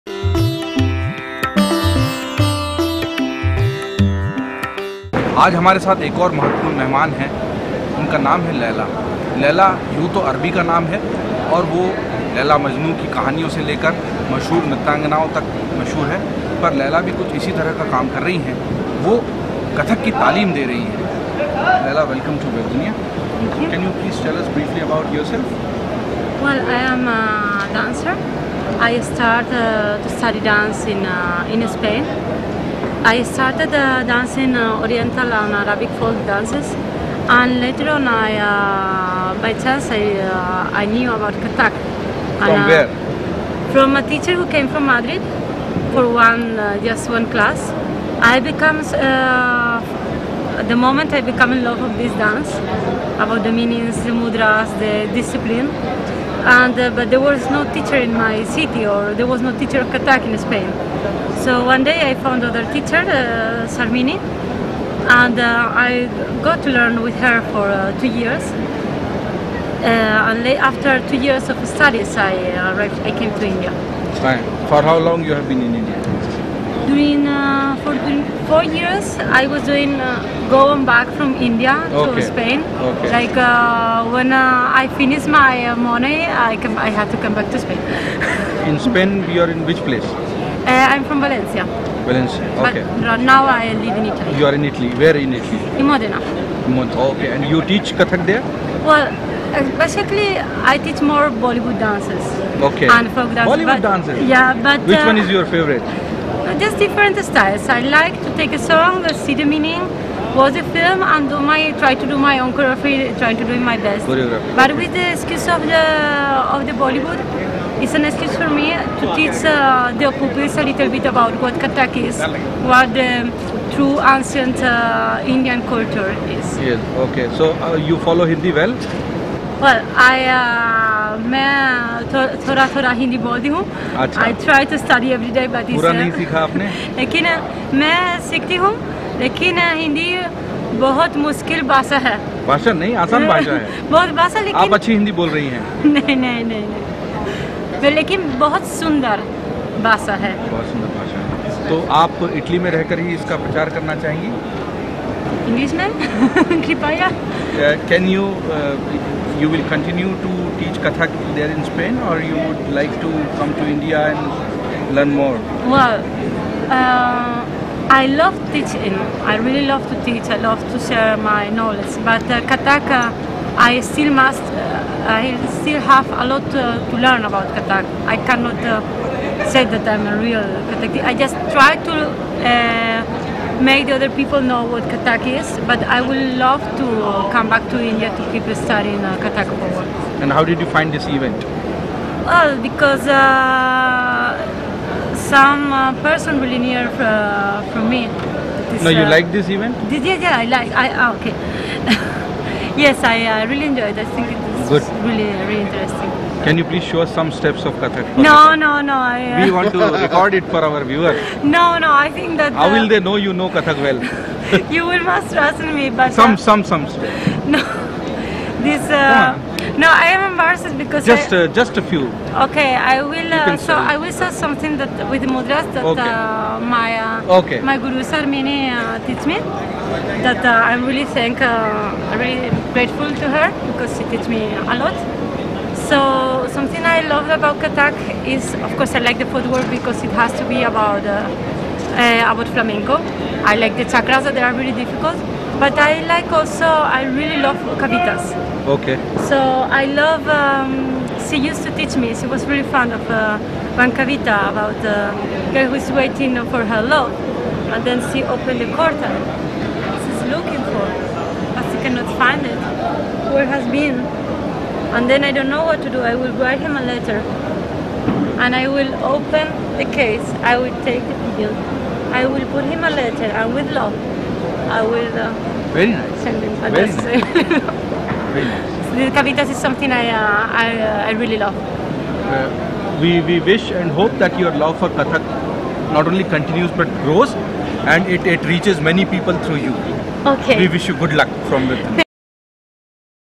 कथक नृत्यांगना लैला : हिन्दी बहुत ही सुंदर भाषा है
स्पेन की रहने वाली लैला कथक नृत्यांगन हैं और अब दूसरों को भी कथक सिखाती हैं। वे कहती हैं हिन्दी बहुत ही सुंदर भाषा है।